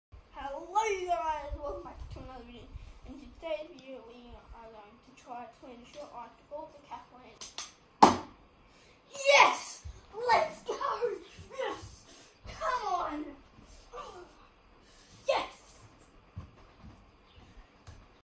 the cap landing bottle flip!!!!!!!!
this i a video of me doing a cap landing bottle flip !!